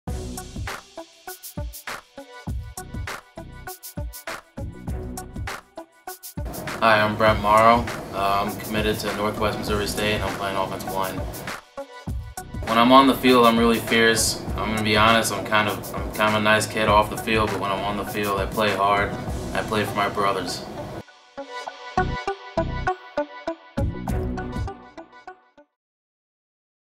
USA Football National Signing Day 2014 | NW Missouri State
Learn more about NW Missouri State's recruits that signed their letter of intent at USA Football's 2014 National Signing Day Breakfast at AT&T Stadium.